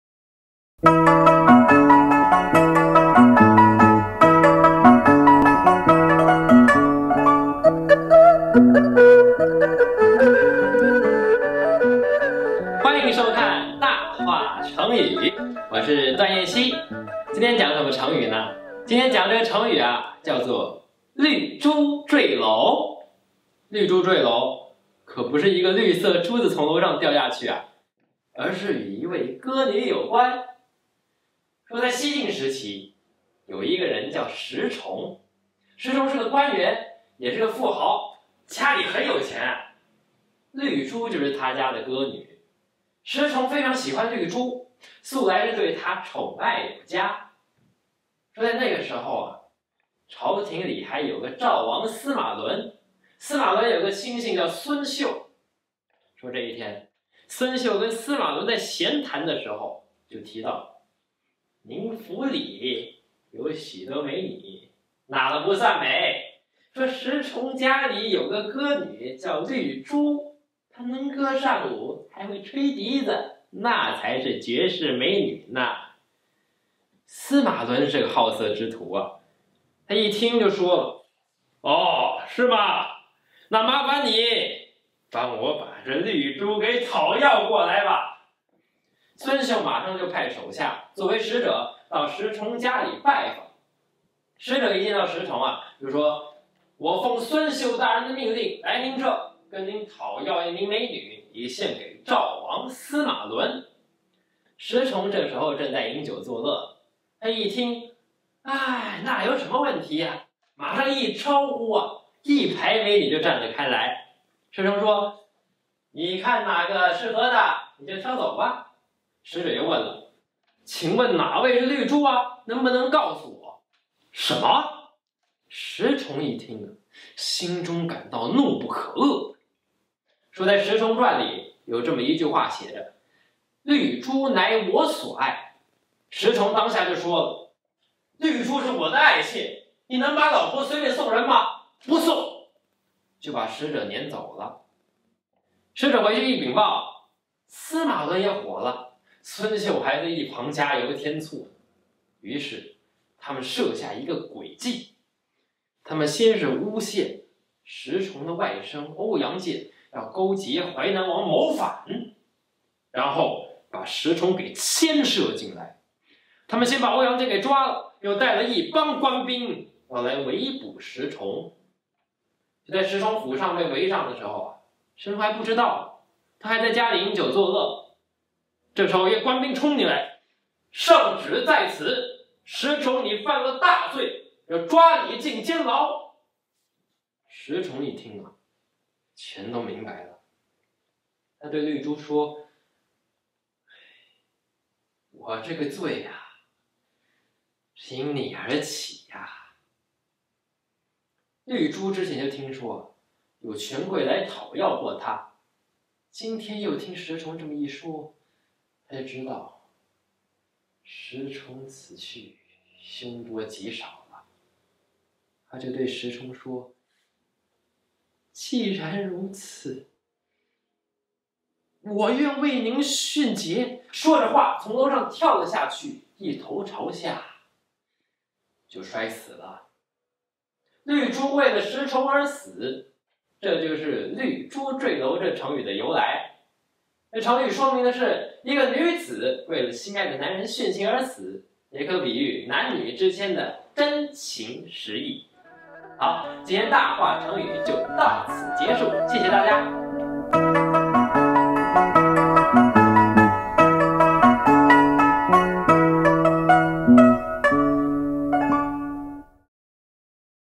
【台北曲藝團】 - 葉怡均評書館 - 大話成語 - 綠珠墜樓 主講 : 段彥希
大話成語,考考你 : 成語 : " 綠珠墜樓 "
綠珠是?" 1. 珍珠 2. 琉璃 3.貓眼石 4.美女